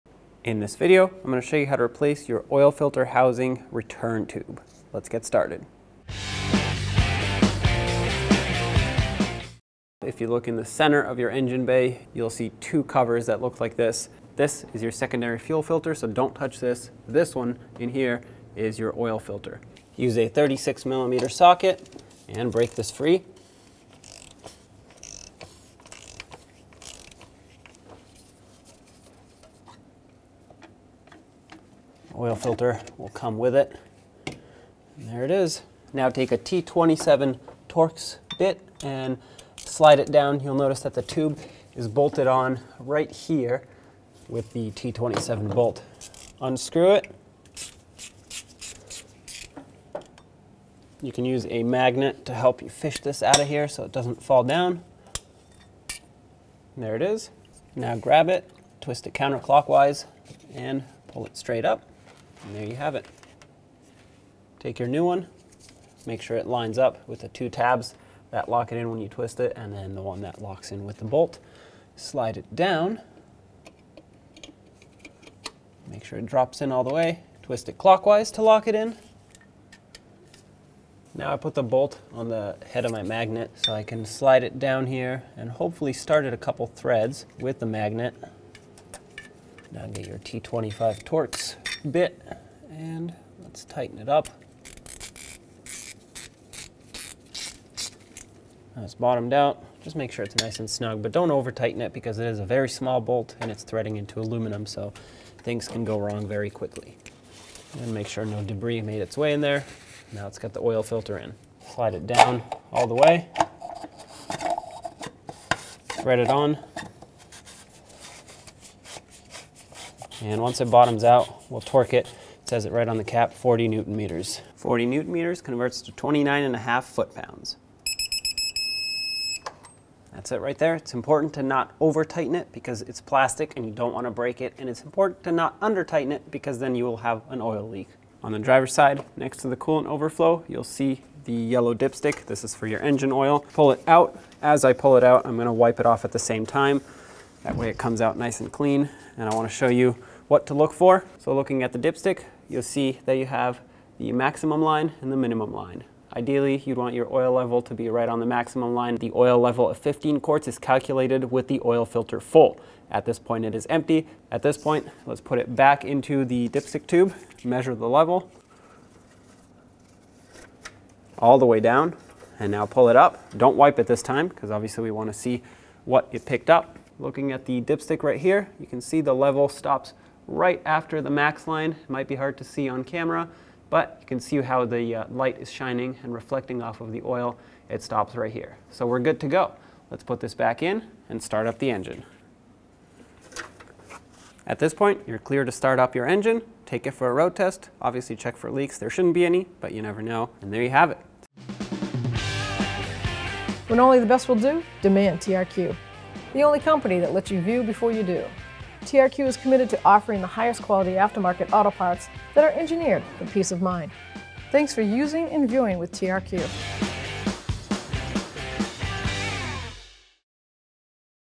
How to Replace Oil Filter Housing Return Tube 2008-2010 Ford F-250 (6.4L Deisel)
This video shows you how to install a new oil filter return tube on your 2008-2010 Ford F-250 (6.4L Deisel).

This process should be similar on the following vehicles:
2008 Ford F-250 Super Duty
2009 Ford F-250 Super Duty
2010 Ford F-250 Super Duty